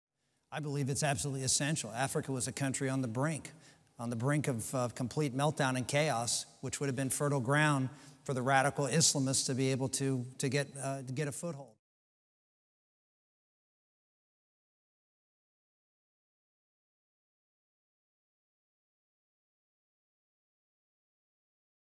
Santorum stumbles, calls Africa a "country"
Touting his work in Africa in the most recent Republican debate, Rick Santorum called the country a "continent." Santorum, who has lagged in the polls since onset of campaign, otherwise performed well in the debate on national security issues.